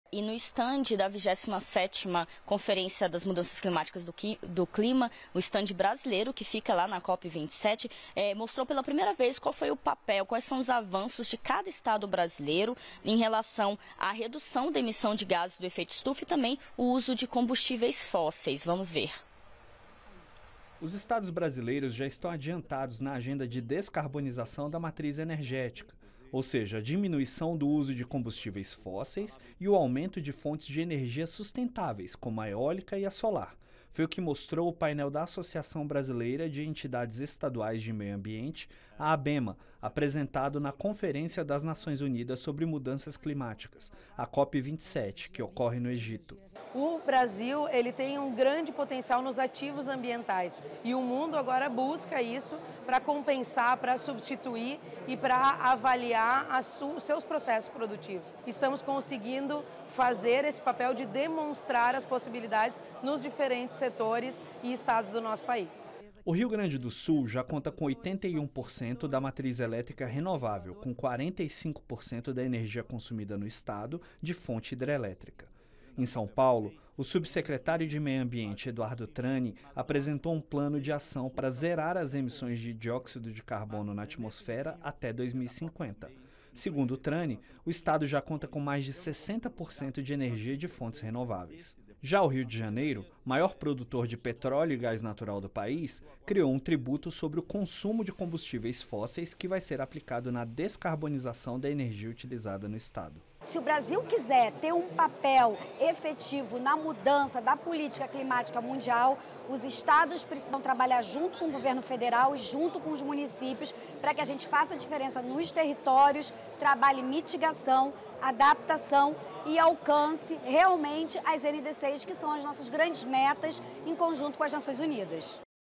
Meio Ambiente | Descarbonização dos estados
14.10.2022 - O estande do Brasil na COP 27 mostrou os avanços de cada estado na redução da emissão de gases e uso de combustíveis fósseis.